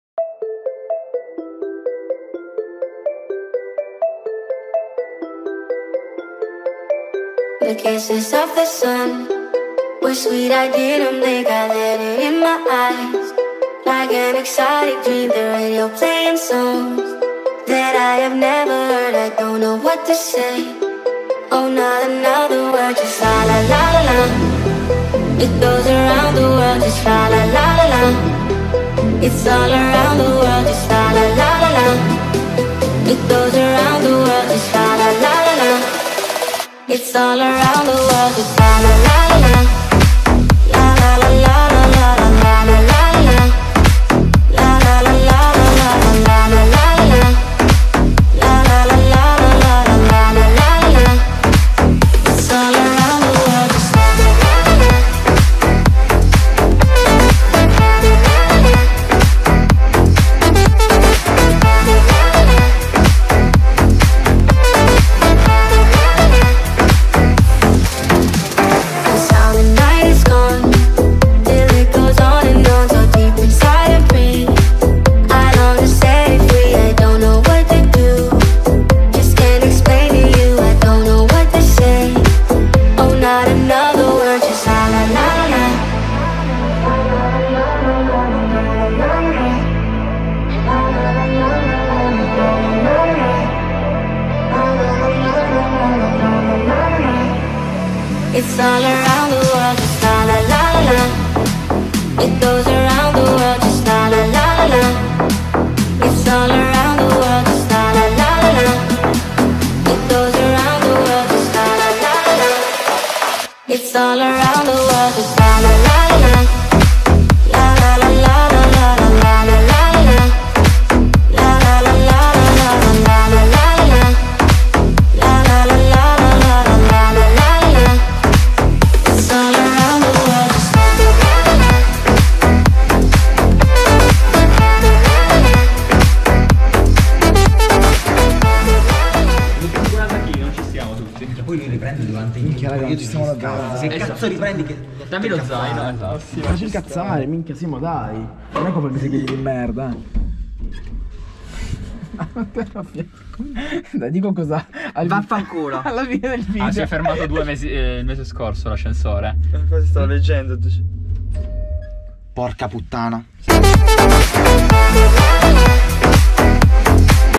{ #7} Mountain Hike – Prali [ 26 / 09 / 2020 ]
Dopo aver iniziato un lavoro abbastanza stressante, in un bel week end con belle temperature (piu' o meno, li si gelava comunque) e del bel tempo si è deciso di andare con un gruppo di amici in montagna a Prali. Ottima giornata e bellissima escursione